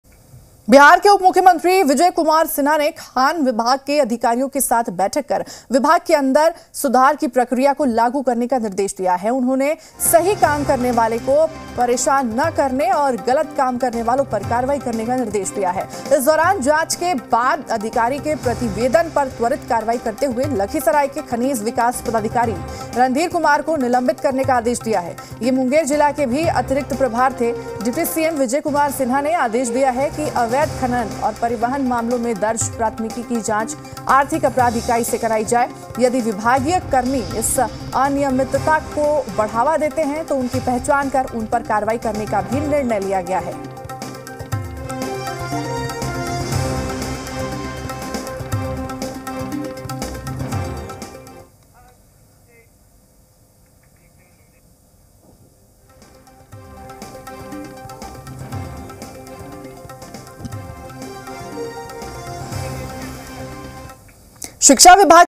Bihar News: Deputy CM Vijay Sinha ने खान विभाग के अधिकारियों के साथ की अहम बैठक
Bihar News: Deputy CM Vijay Sinha ने खान विभाग के अधिकारियों के साथ की अहम बैठक...देखिए पूरी वीडियो...